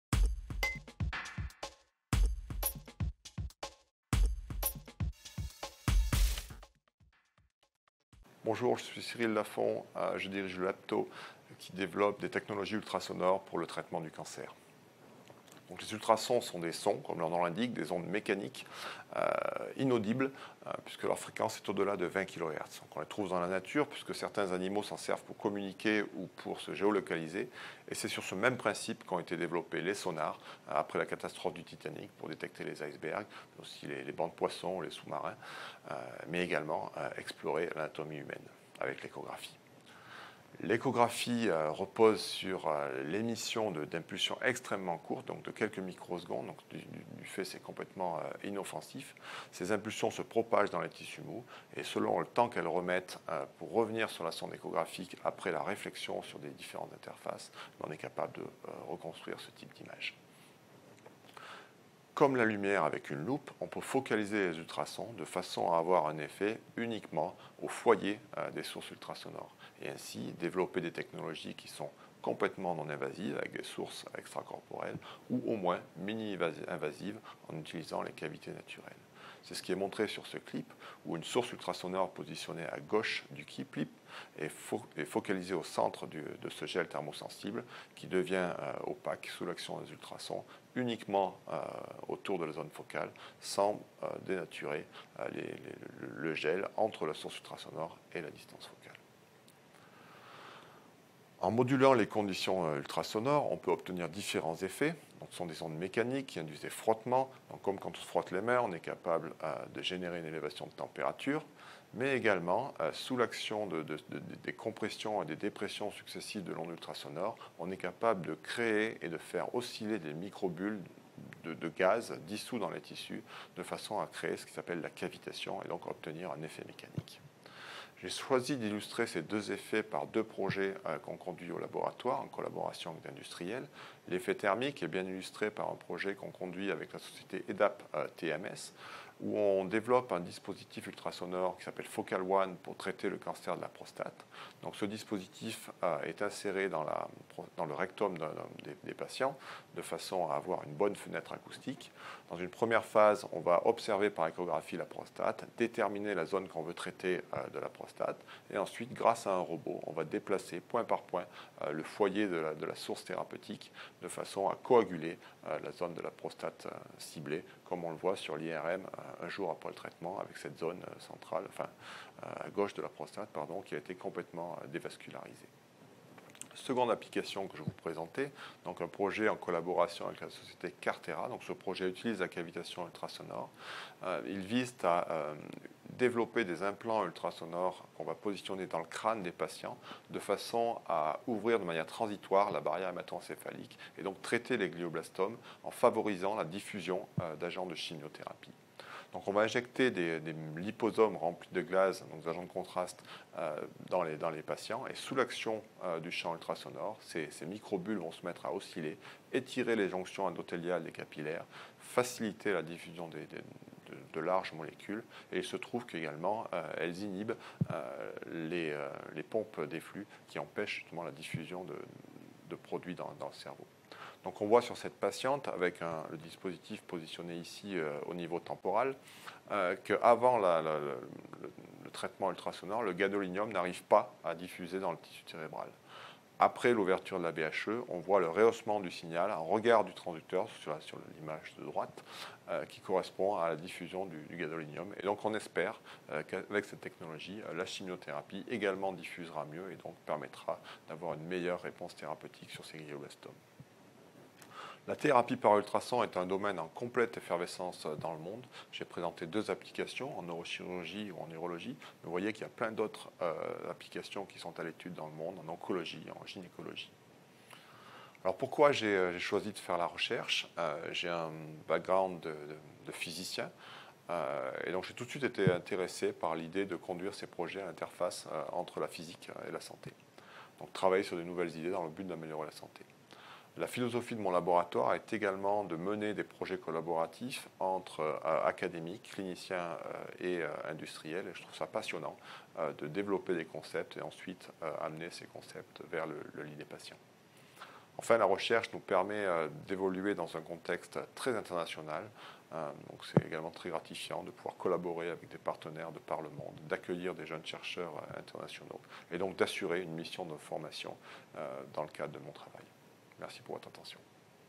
Une recherche, un chercheur - Cyril Lafon, les ultrasons contre le cancer
Une recherche, un chercheur : une série vidéo où des chercheurs de Lyon 1 présentent un point particulier de leur recherche, à quoi ça sert dans la vie de tous les jours et pourquoi ils ont eu envie de devenir chercheur. Le département de Biologie Humaine à l’origine du projet présente la deuxième vidéo : Cyril Lafon, directeur du LabTAU, nous fait découvrir sa recherche à l’interface de la physique et de la santé. Comment les ultrasons peuvent être utilisés dans le traitement contre le cancer ?
Réalisation service ICAP – Université Lyon 1. Tournage en studio vidéo SECA
Solaine Reynaud, développement et suivi du projet / équipe vidéo
Fanny Prémaillon, développement et suivi du projet / équipe vidéo
Julien Manganoni, montage / équipe vidéo
Auteur : Camille Navet – Département de Biologie Humaine – Université Lyon 1